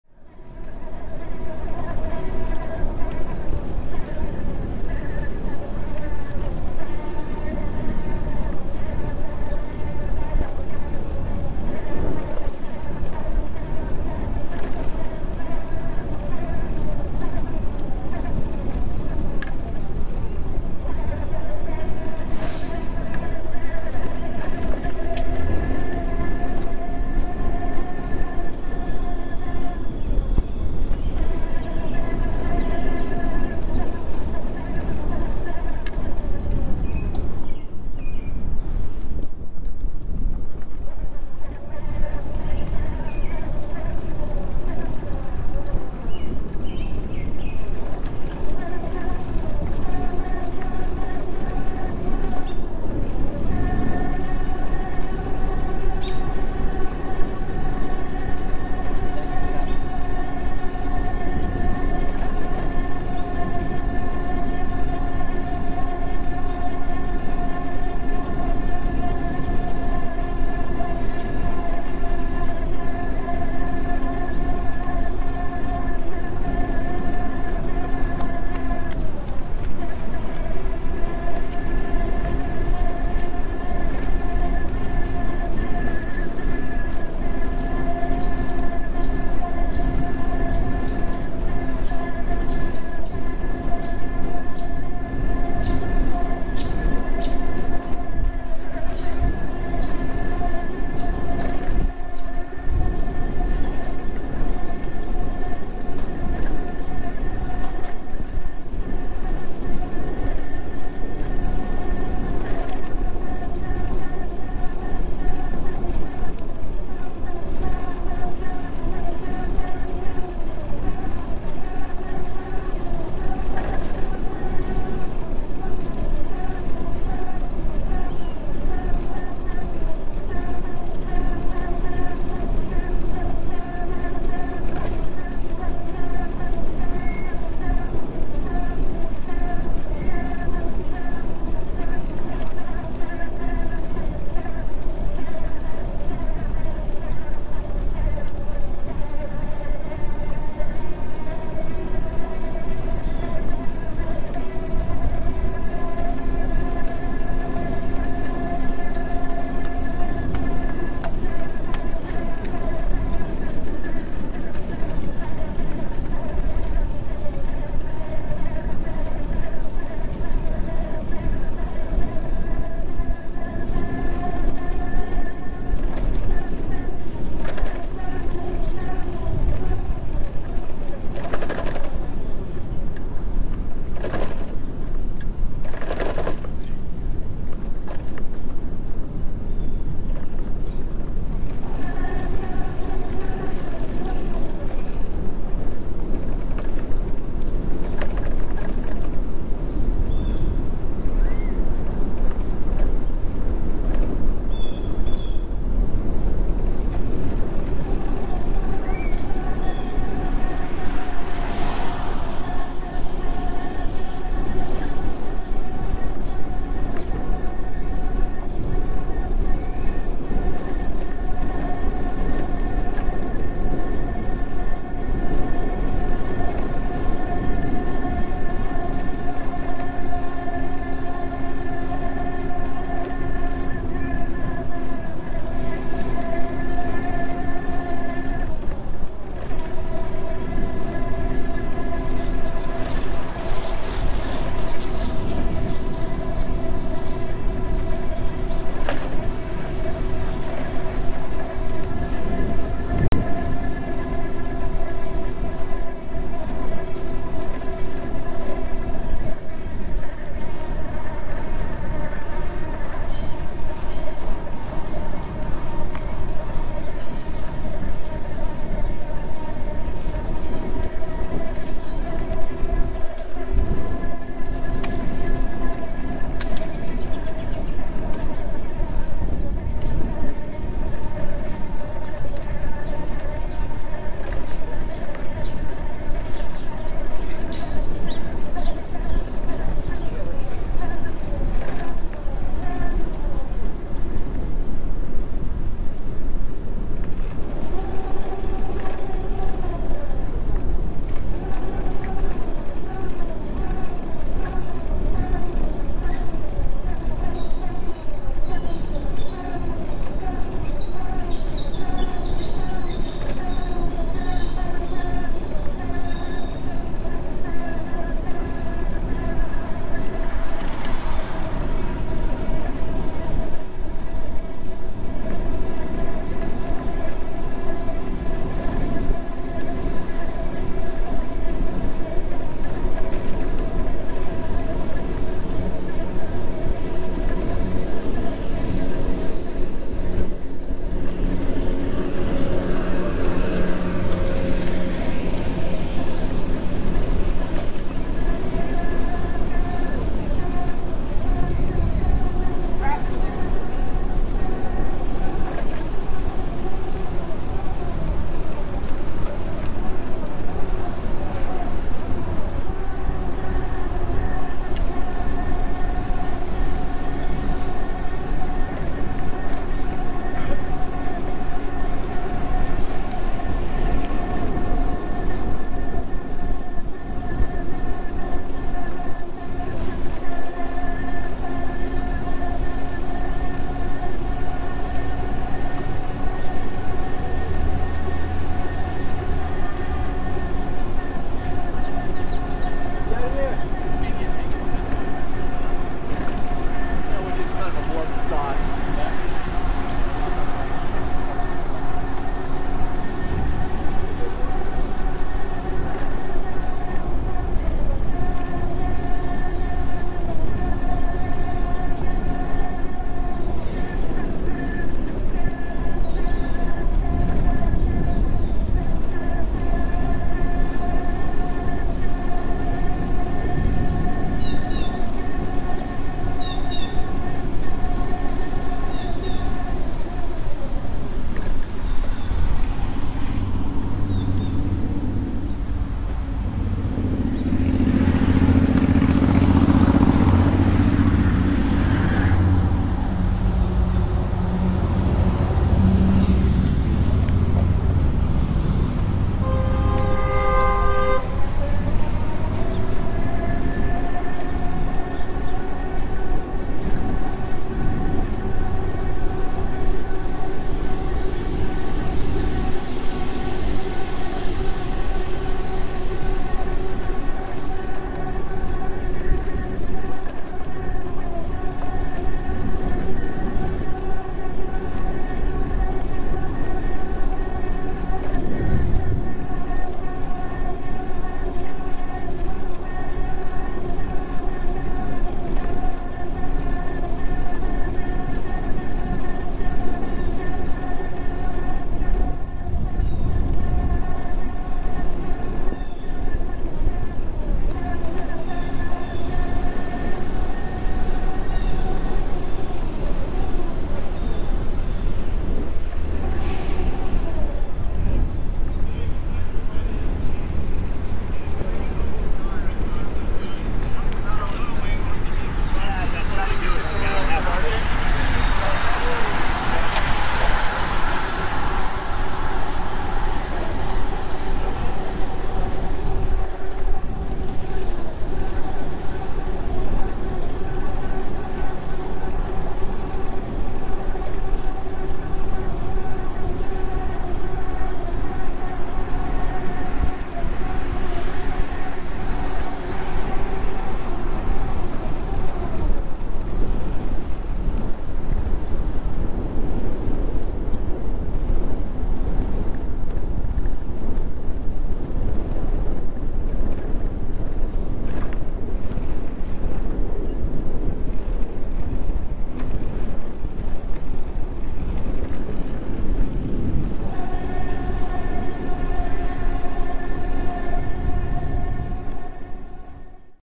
Central Park is a great place to ride a bike in Manhattan | NYC | Binaural Audio | 4K
This video was recorded in August 2021, cycling from Central Park South to W 110th St via East Drive. Enjoy!